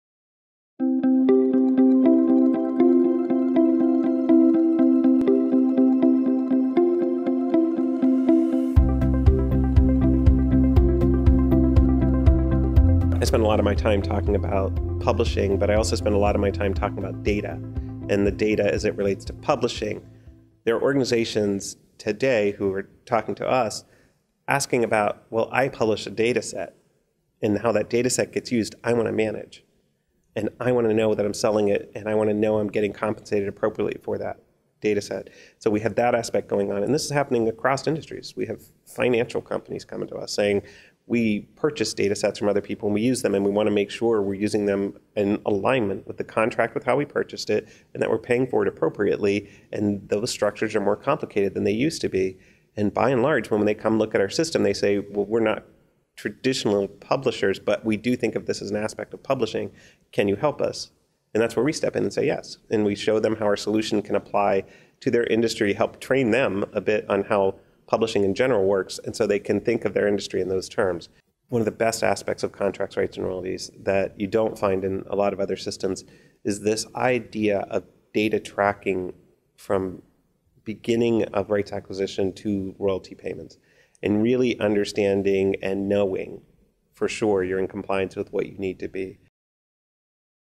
Contracts Rights and Royalties - How does it help organisations?
How does the Contracts, Rights and Royalties system benefit organisations? It's all about effective management of data. Drive your rights value and royalties compliance directly from your contract metadata.

The Contracts Rights and Royalties publishing software is part of the Ingenta Commercial suite. It can be purchased and implemented on it's own and benefits from being content agnostic. The platform helps publishers with intellectual property management and is an ideal book management solution for scholarly publishers as well as other content providers. The product is used globally as a rights management software and royalty management software helps publishers remain copyright compliant.